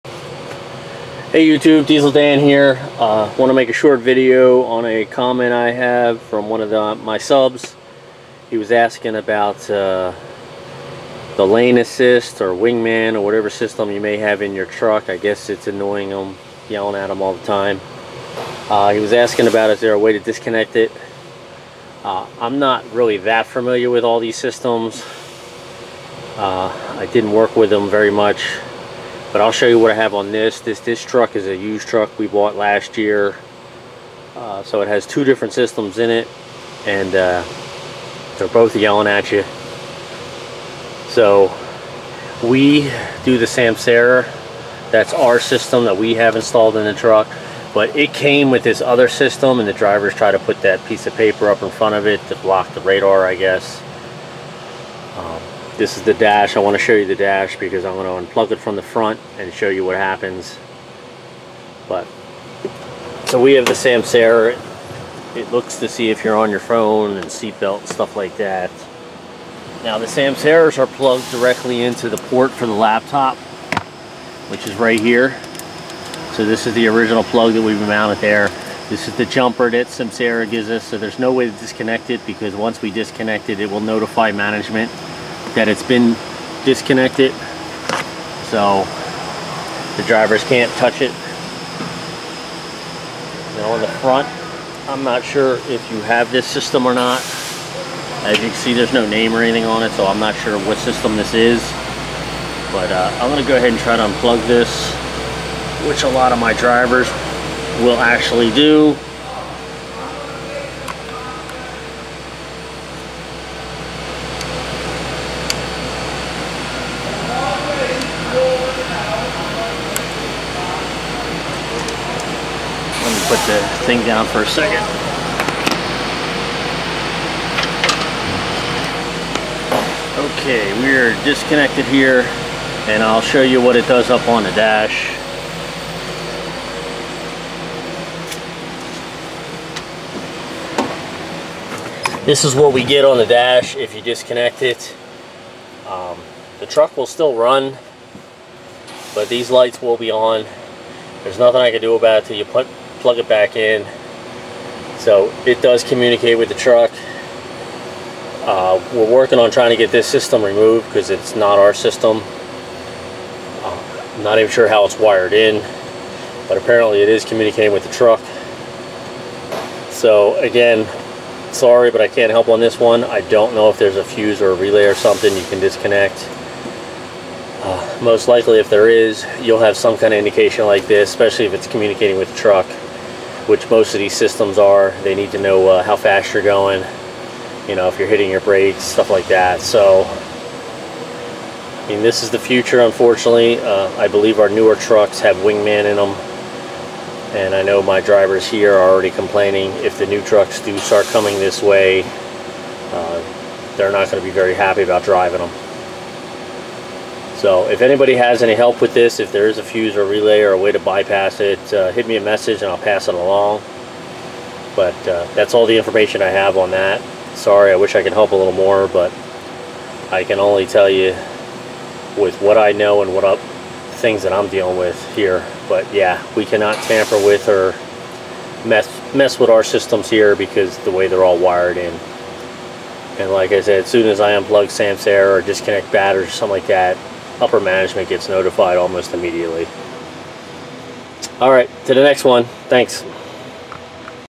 How to bypass drive cams in Semi trucks
Sorry guys there is no way to bypass the drive cams in the trucks, they're wired into the truck where they cannot be tampered with